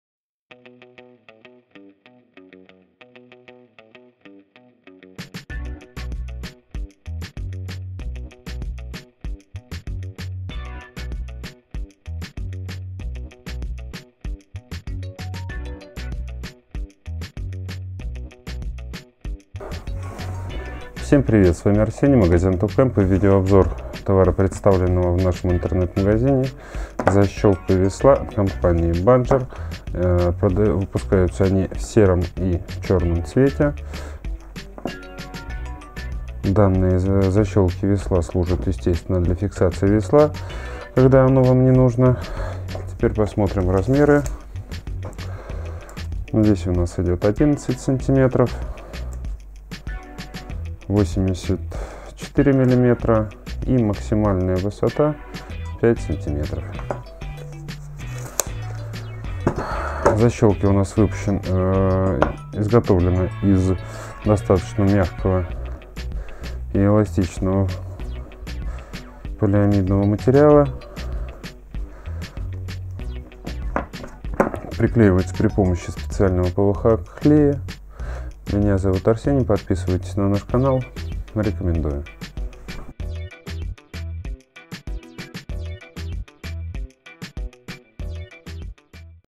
Защелка весла
Защелка для крепления весла выполнена из материала PVC, в ней фиксируется весло во время движения под мотором или на время остановки. Само крепление представляет из себя прямоугольный PVC пятак размером 85х110 см, с выступами для фиксации веслами. Для того, что бы приклеить крепление к лодке вам понадобится клей ПВХ, который вы можете приобрести так же у нас в интернет магазине.